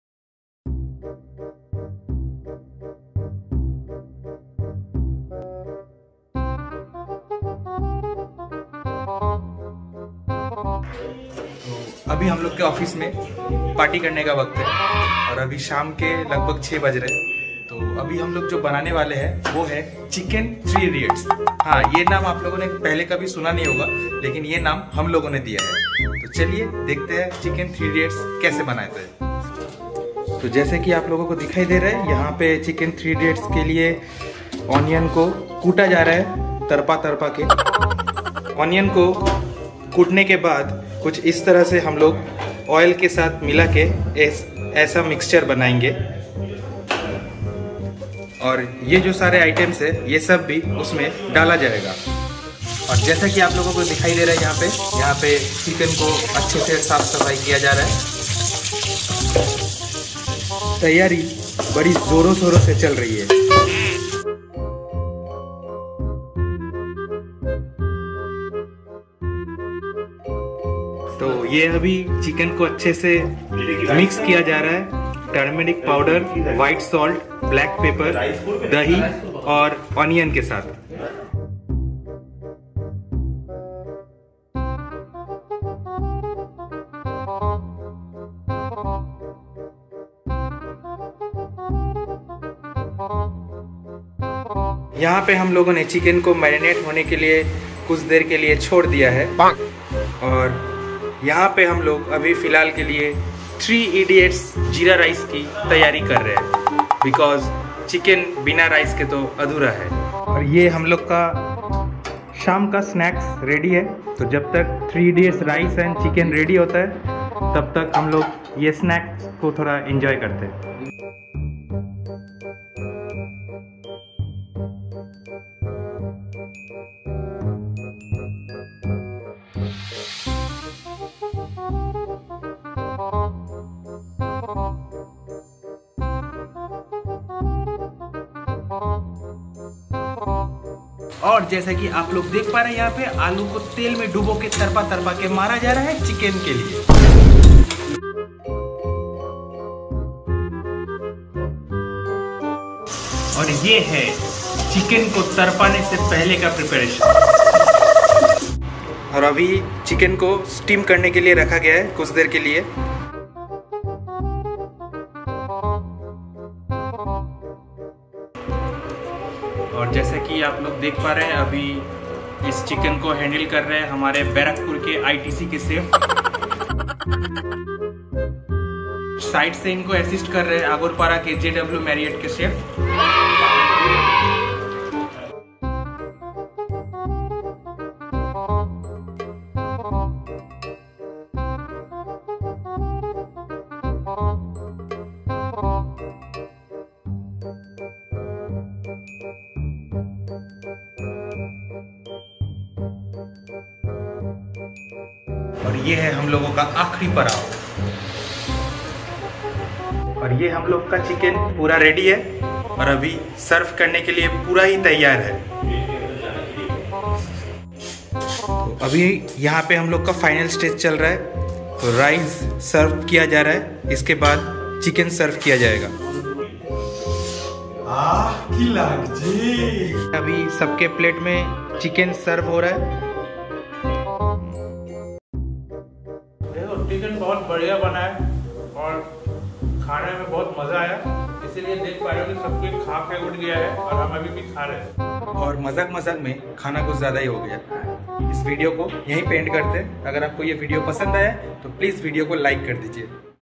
Chicken Recipe | Office Kitchen | Cooking Class 😁
Office Kitchen Episode -1

Watch it till the end if you want to know how to make chicken kasha in Office and what are the basic ingredients you need.